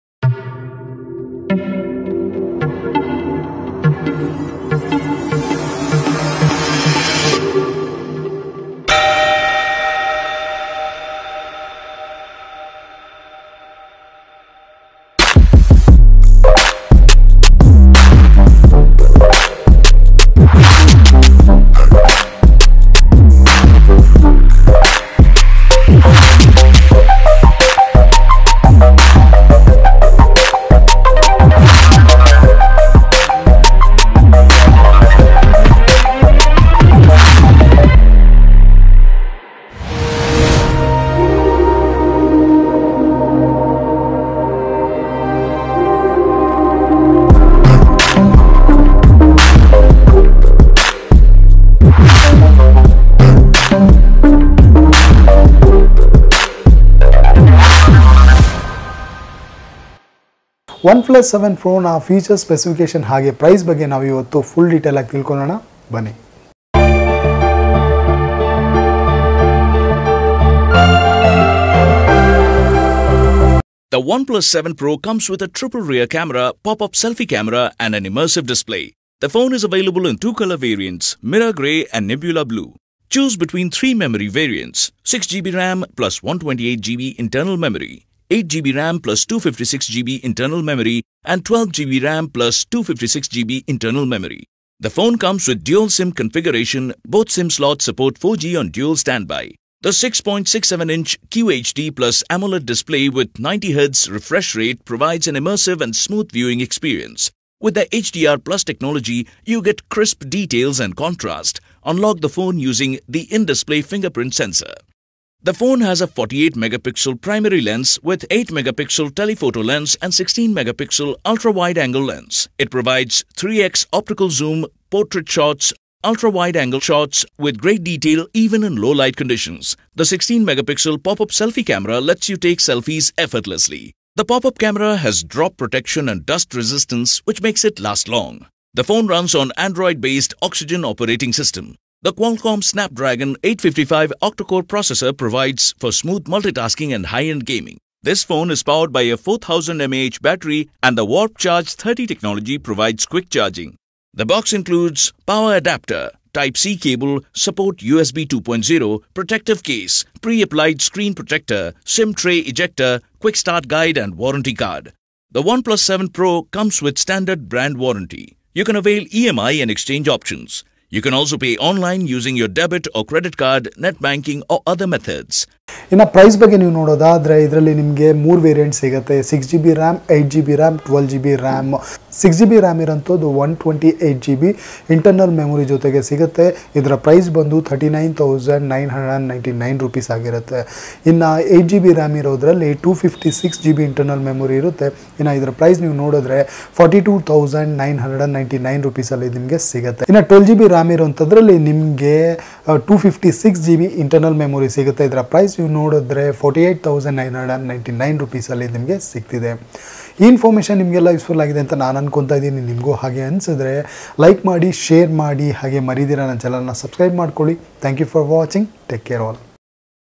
OnePlus 7 Pro in Kannada, Features and Specifications
Rear Camera|48MP (Primary)+ 8MP (Tele-photo)+16MP (ultrawide)| Front Camera|16 MP POP-UP Camera
16.9 centimeters (6.67-inch) multi-touch capacitive touchscreen with 3120 x 1440 pixels resolution
Memory, Storage and SIM: 8GB RAM | 256GB internal memory | Dual SIM dual-standby (4G+4G)

Android Oxygen operating system with 2.84GHz Snapdragon 855 octa core processor

4000mAH lithium-ion battery, Buttons: Gestures and on-screen navigation support; Alert Slider

1 year manufacturer warranty for device and 6 months manufacturer warranty for in-box accessories including batteries from the date of purchase

Box also includes: Power Adapter, Type-C Cable (Support USB 2.0), Quick Start Guide, Welcome Letter, Safety Information and Warranty Card, Logo Sticker, Case, Screen Protector (pre-applied) and SIM Tray Ejector